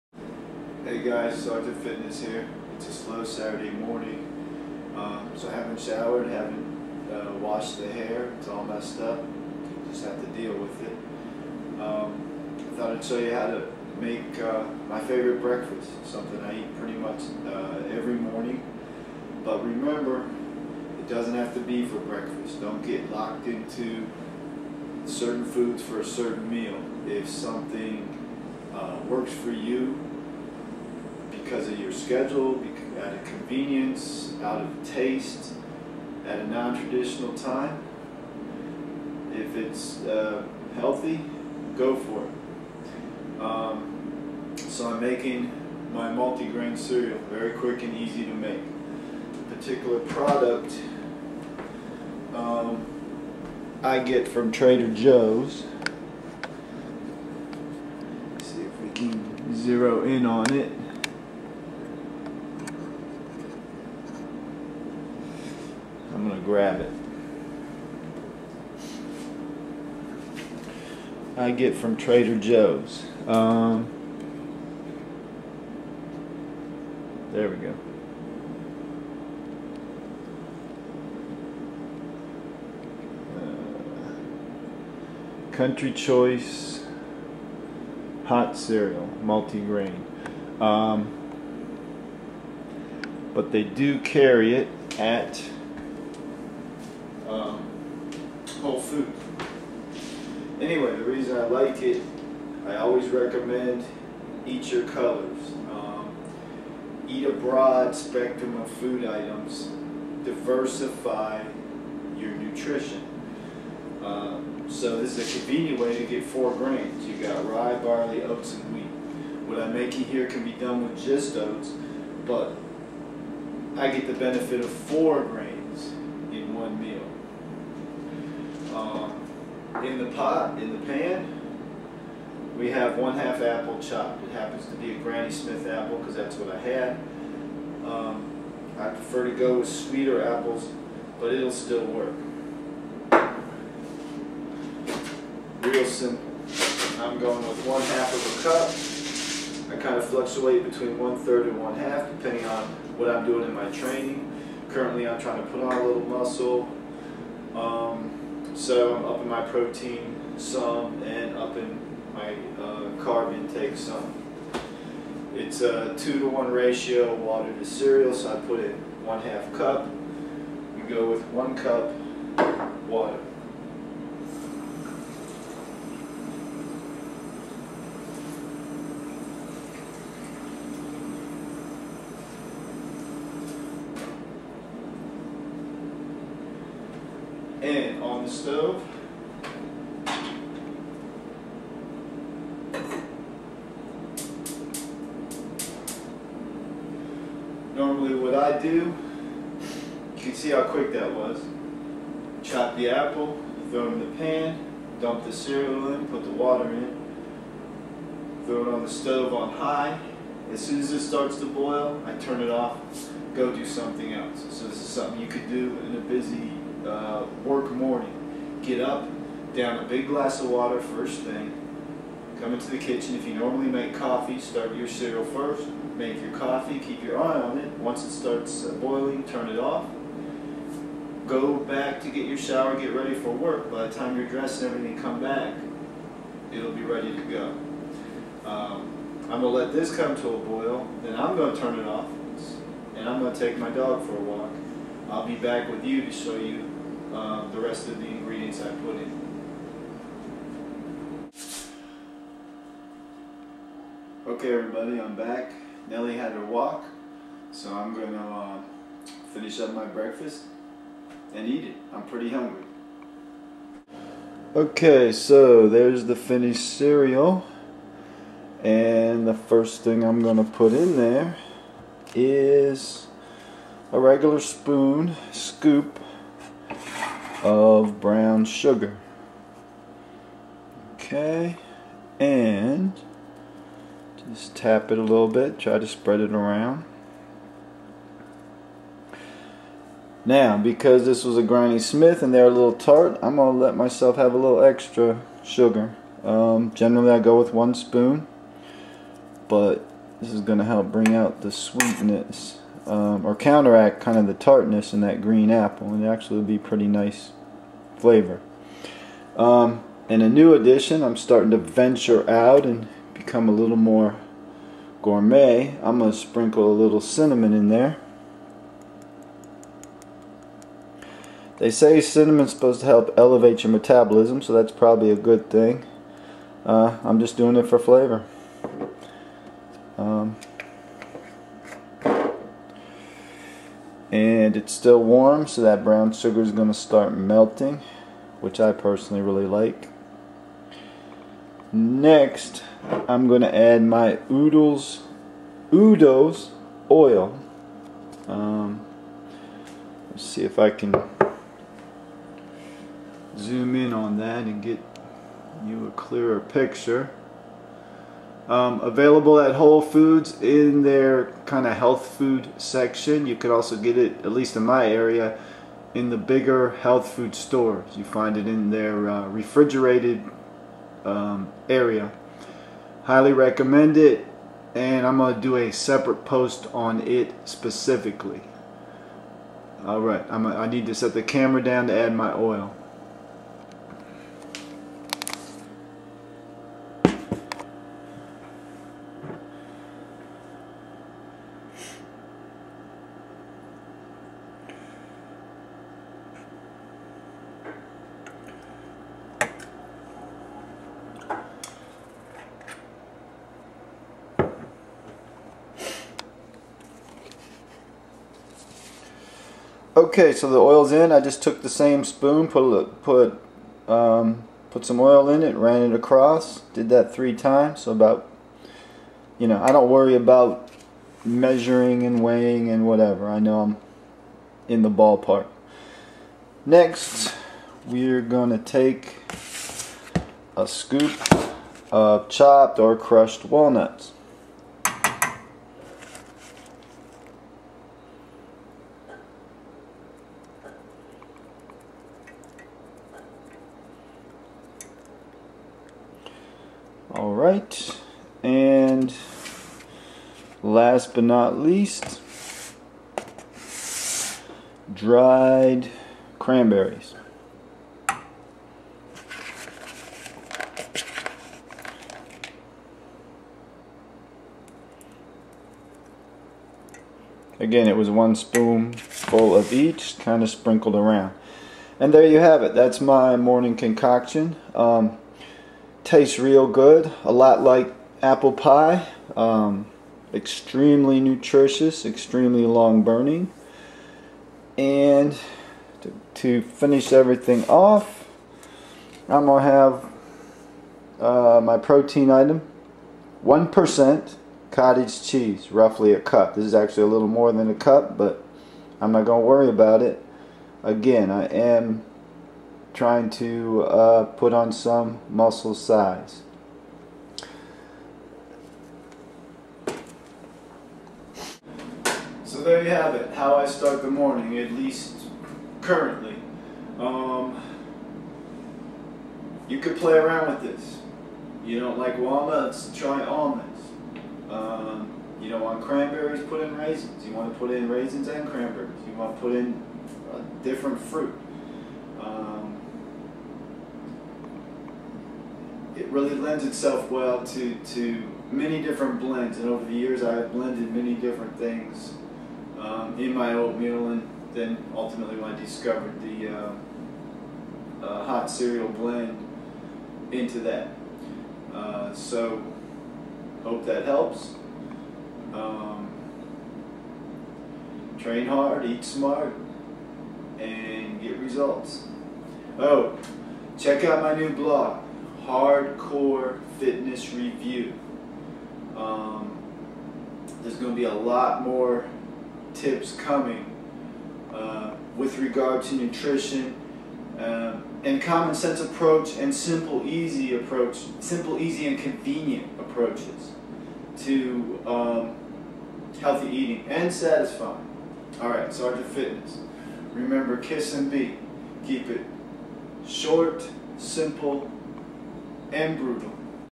A quick and easy healthy breakfast
A super healthy, quick , and easy breakfast that will keep you reeving all day long.